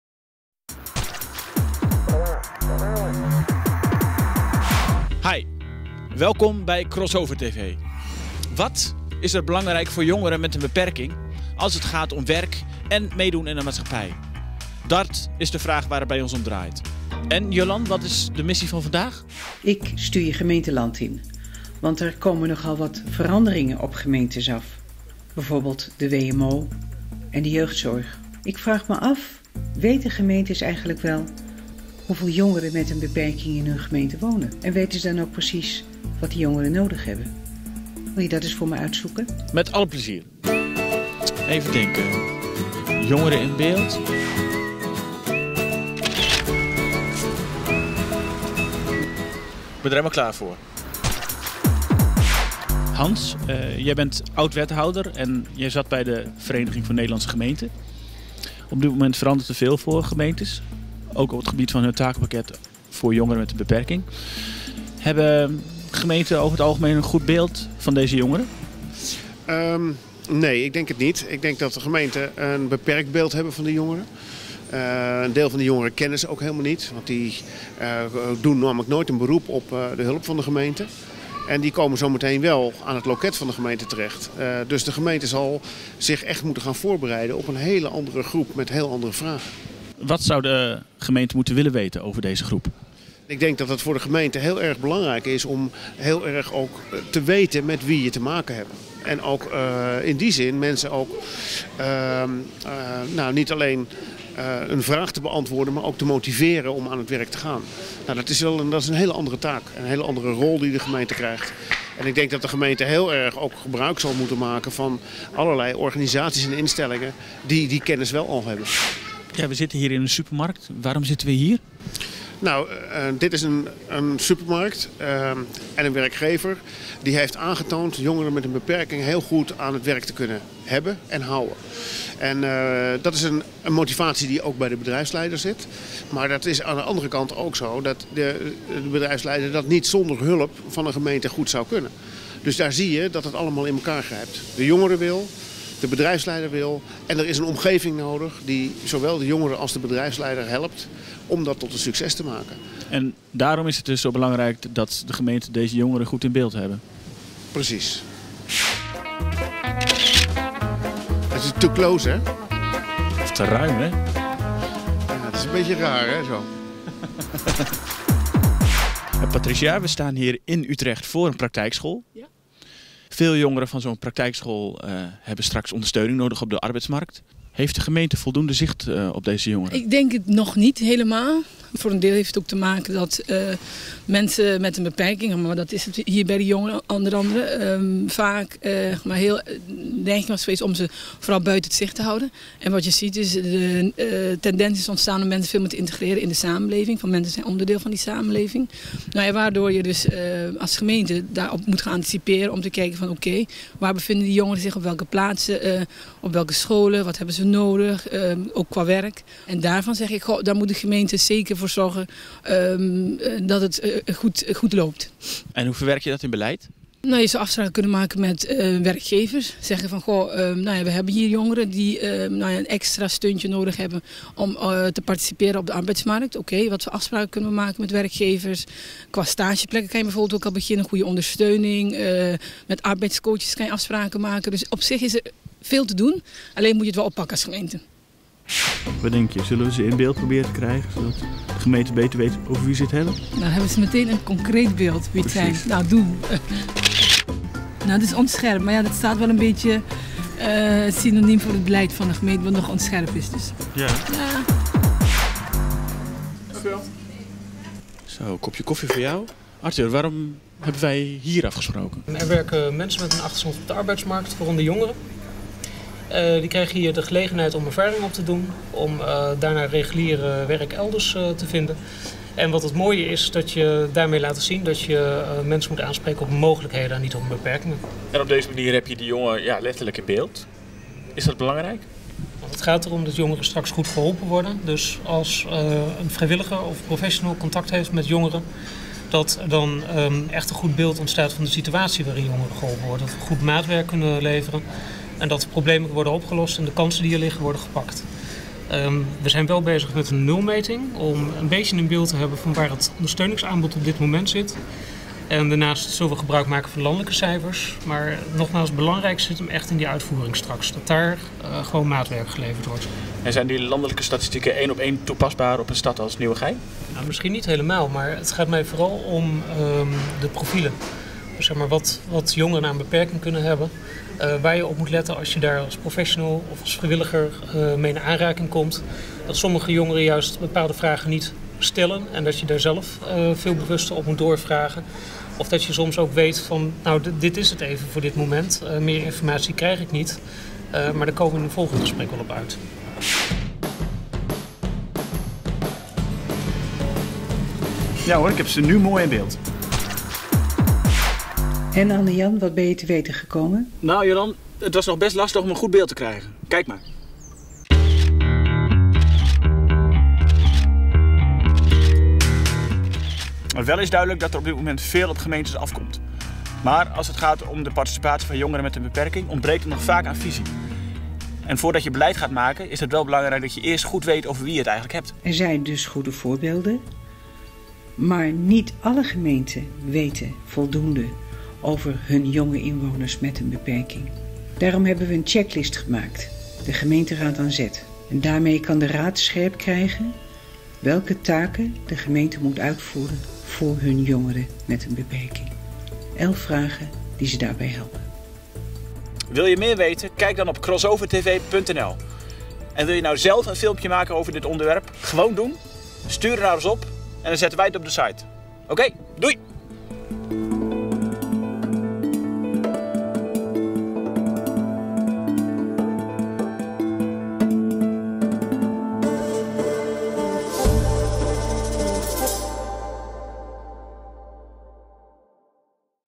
Afl 6 Gemeente
Deze aflevering gaat over de rol van gemeenten. Er komen nogal wat veranderingen op gemeenten af, in de jeugdzorg bijvoorbeeld. Steeds meer verantwoordelijkheden voor jongeren met een beperking komen bij de gemeente te liggen. Maar: hebben gemeenten deze groep wel goed in beeld? Weten ze welke jongeren in hun gemeenten wonen en wat zij nodig hebben? Dat gaan we uitzoeken in deze aflevering!